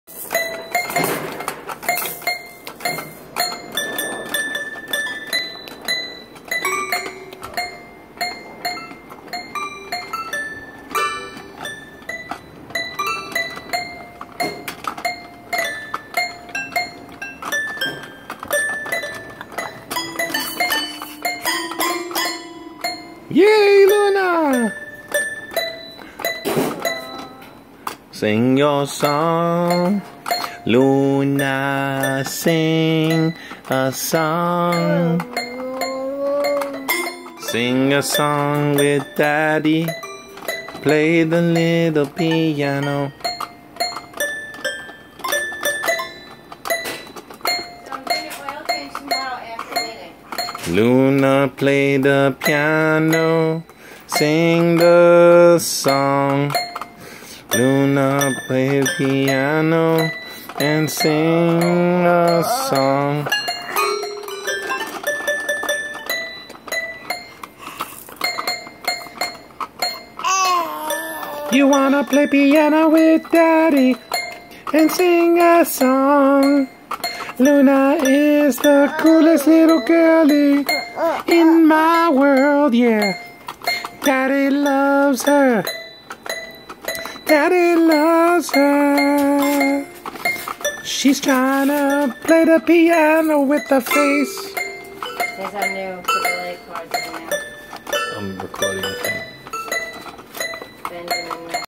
Pink Piano Progression.... almost 8 months
Luna Is starting to play w individual fingers not just banging on the keys. She loves sliding her maracas across the keys. She starts singing in this video too. ;)